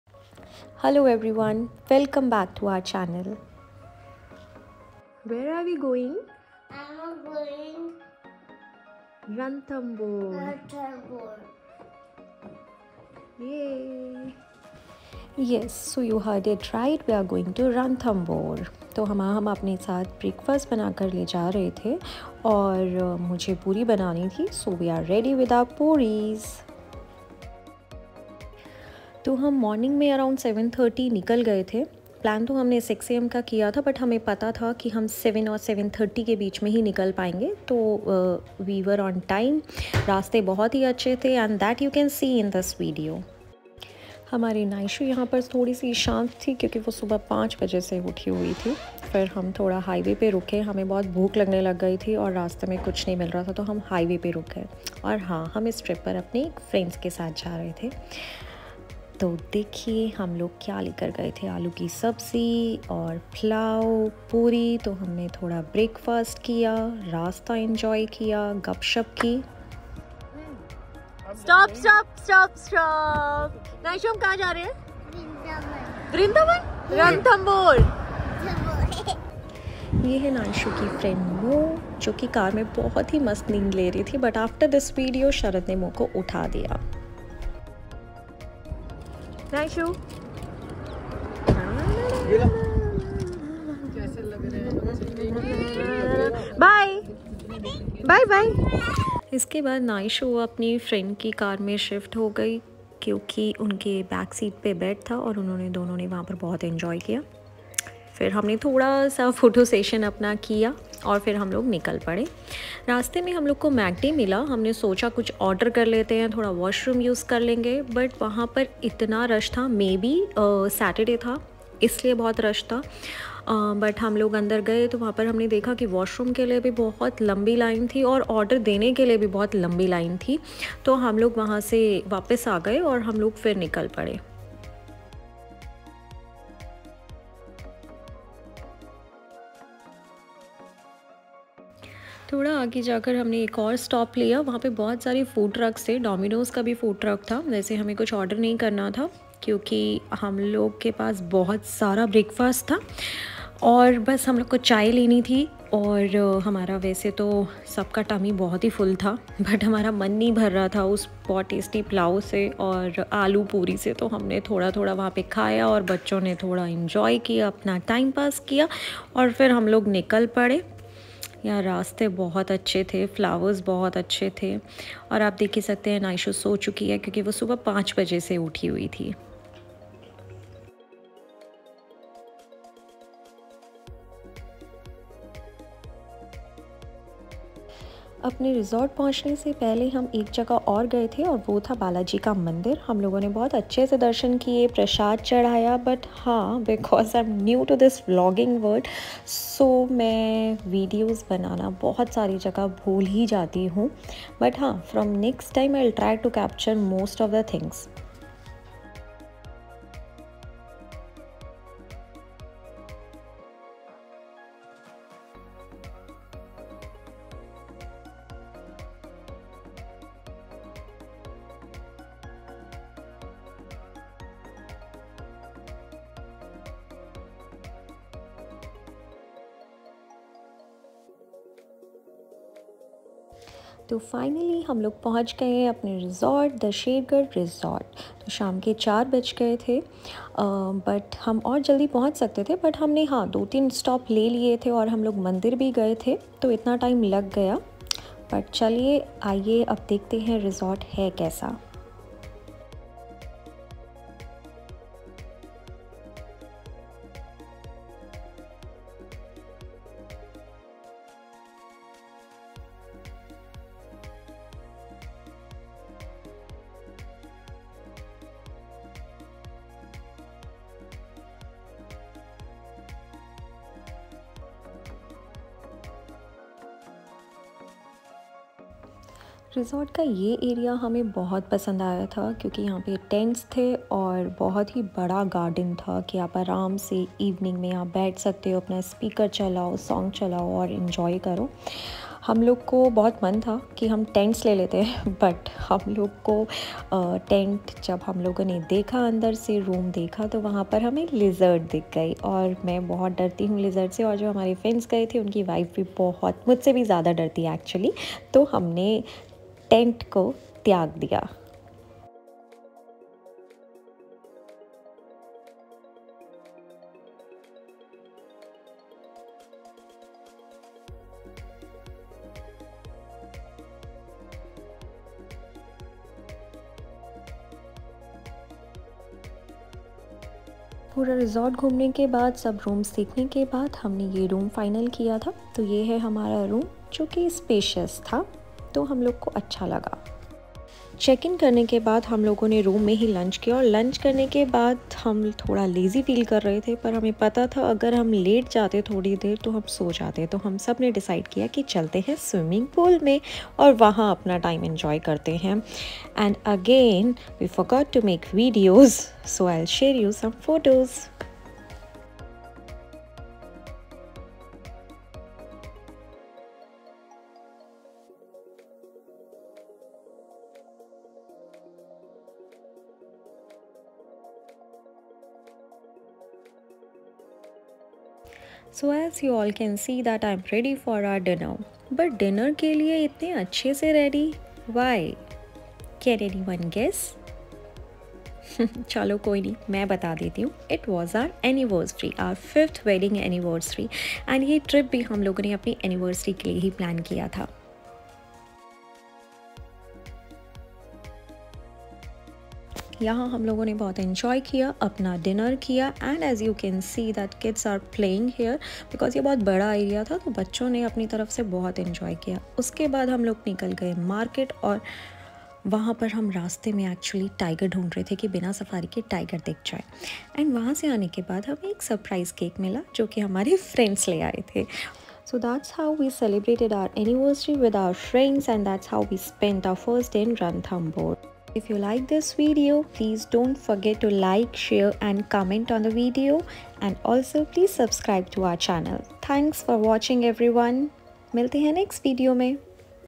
DAY 1 - Ranthambore tour|Trip to Ranthambore|Gzb to Ranthambore via Mumbai Exp.way
For best stay in Ranthambore check out Sher Garh Resort